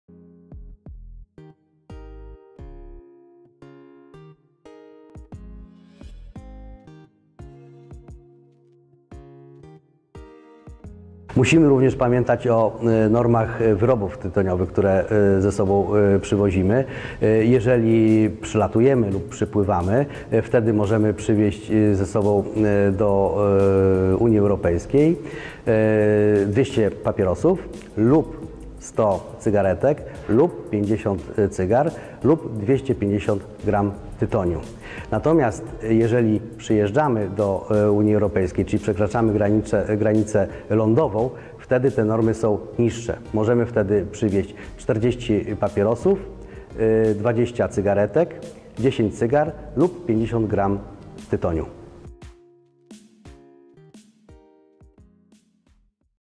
Poradnik wakacyjny MF i KAS - normy w przewozie wyrobów tytoniowych
Trzeci odcinek naszego poradnika wakacyjnego dotyczy przewozu wyrobów tytoniowych. Zachęcamy do obejrzenia!